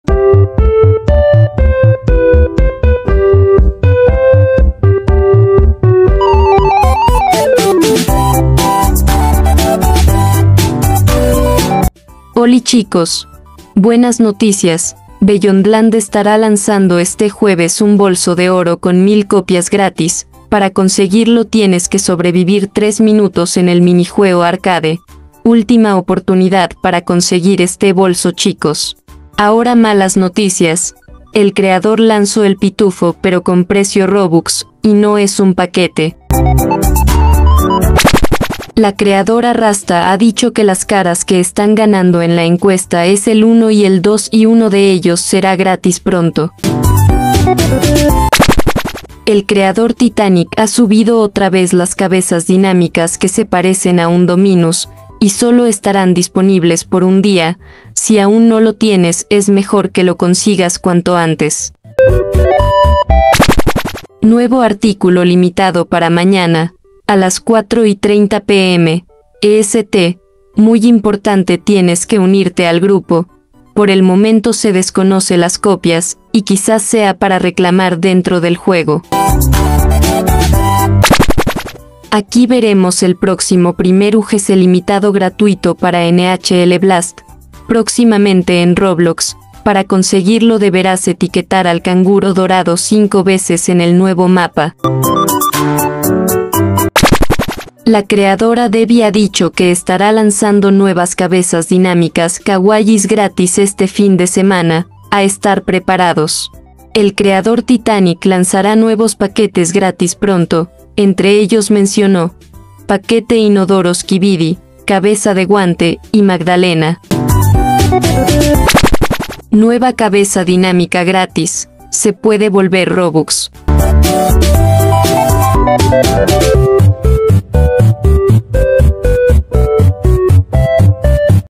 NUEVO ACCESORIO BOLSO DORADO DE BEYONDLAND, CABEZAS DINÁMICA DOMINUS VOLVIERON, NOTICIAS DE ROBLOX
☑️NUEVO LIMITED CAPUCHA

☑️CABEZAS Y CUERPOS DINÁMICOS GRATIS, SE PUEDEN VOLVER ROBUX